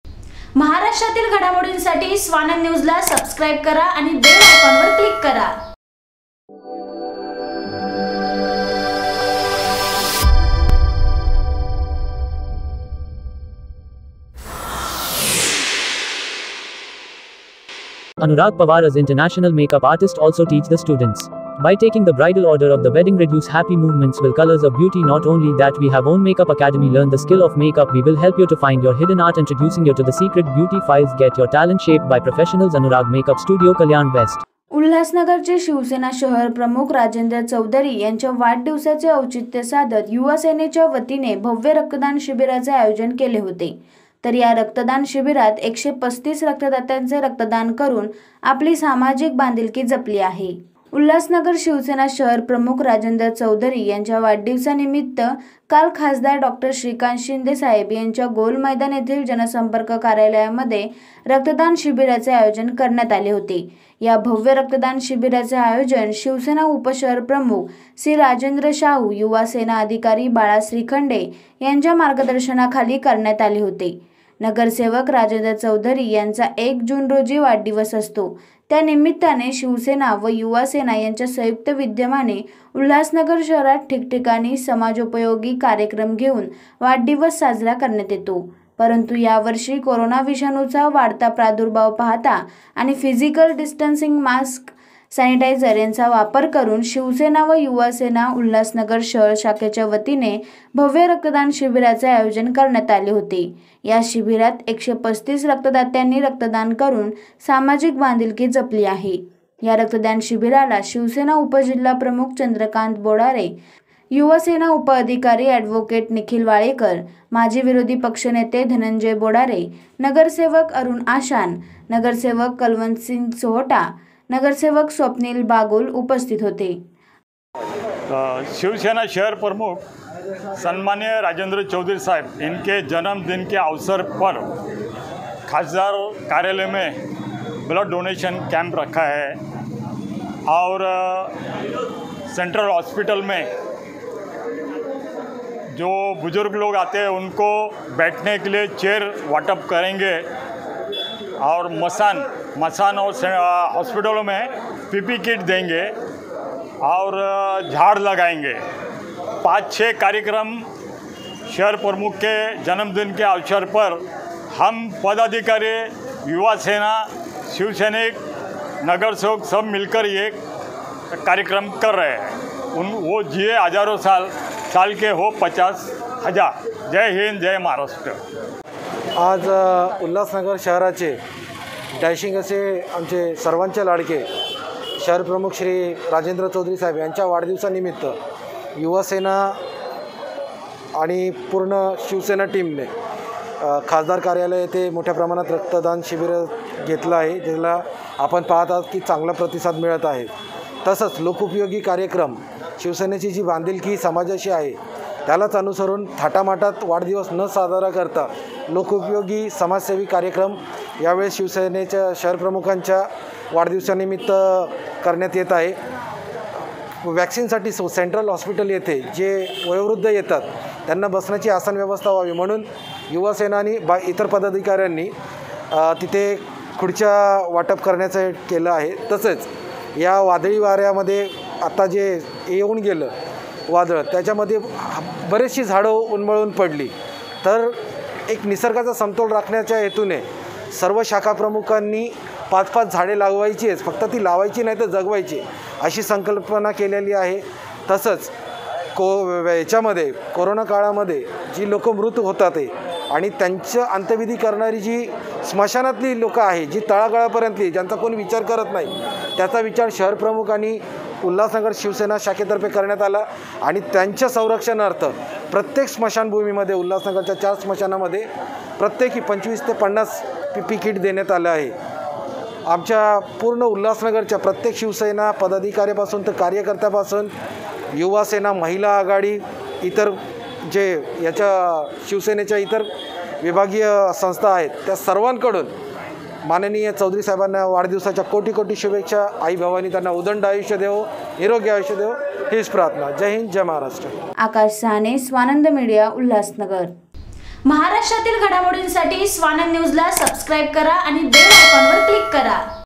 युवासेनेच्यावतीने भव्य रक्तदान शिबीर संपन्न ! शहरप्रमुख राजेंद्र चौधरी यांच्या वाढदिवसानिमित्त आयोजन
SEND ME STUFF:

Near B.K.No.1416,
Maratha section - 32,
Ulhasnagar - 421004,
Dist. : Thane,
Maharashtra,
India